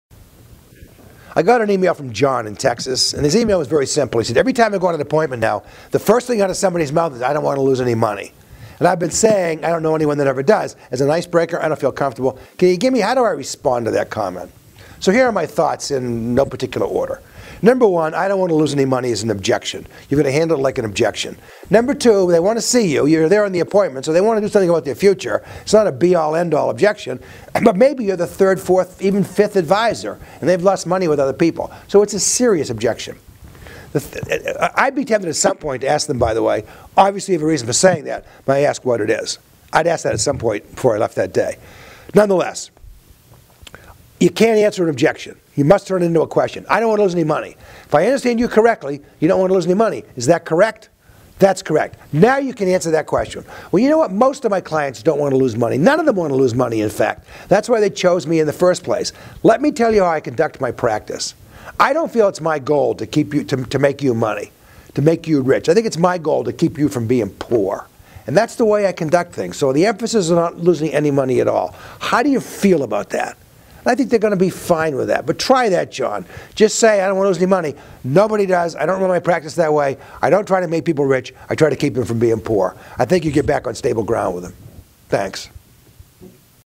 How to respond to the comment "I don't want to lose any money" - AskDON Episode
Watch this video episode or read the transcript to learn Don Connelly’s answer to the comment "I don't want to lose any money" - a common client objection when it comes to long term investing.

– a learning center for Financial Advisors with hundreds more audio and video podcasts that will help you develop superb soft skills for success.

- delivered every Tuesday via email for FREE